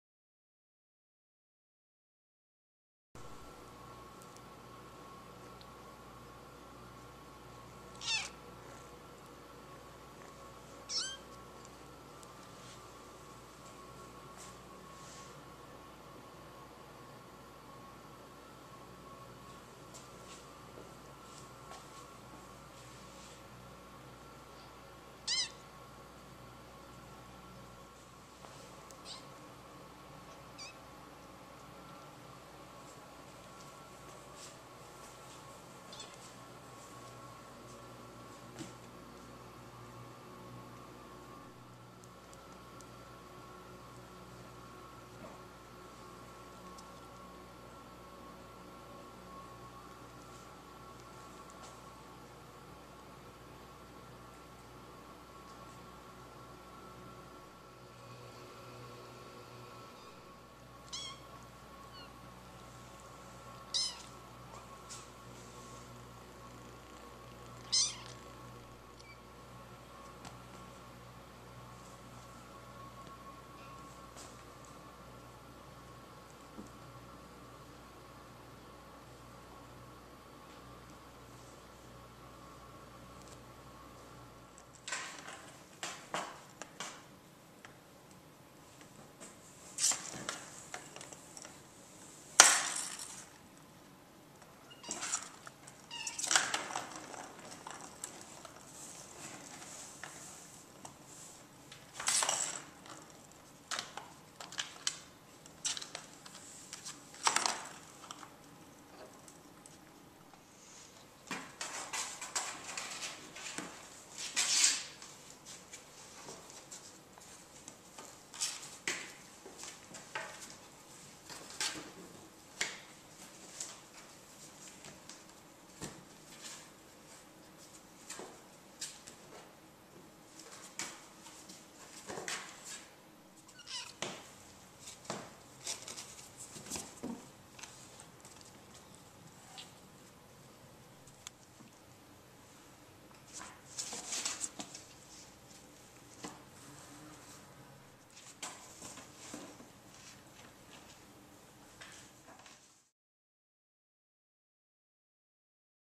New KAAP Kittens ready to be picked for adoption Nov 11 2012
These sweetie pies are less than 8 weeks old right now. Two Calico girls (one looks like a Torti but has a white belly, so technically a Calico), and two shiny black boys. They lost their mom at 4 weeks of age, and have been bottled fed and now weaned. Bottle fed kittens are always so snuggly. All they want to do is snuggle in your lap and purr and sleep. These kittenes will be vet checked and vaccinated in a week, but they are available to meet and be picked prior to that time. They can be adopted after they are vaccinated. KAAP adoption fee of $175 covers their first vaccinations, their spay/neuter and tattoo at a later date.